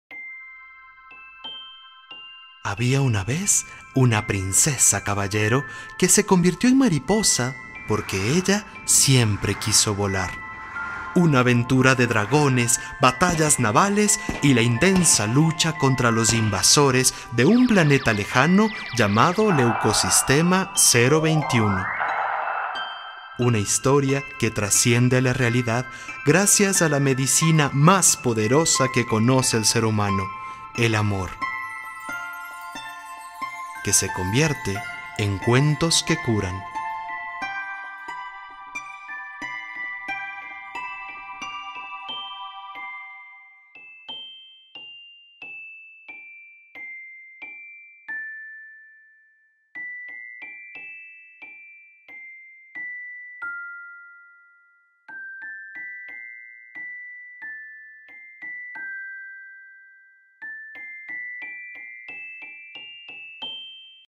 Booktrailer "La princesa Caballero", primer cuento sobre cáncer infantil.
"Y la princesa Caballero se convirtió en mariposa, porque ella siempre quiso volar" ...
Una historia de amor que cuenta las aventuras que una niña de 5 años vive mientras transcurre su tratamiento contra el cáncer.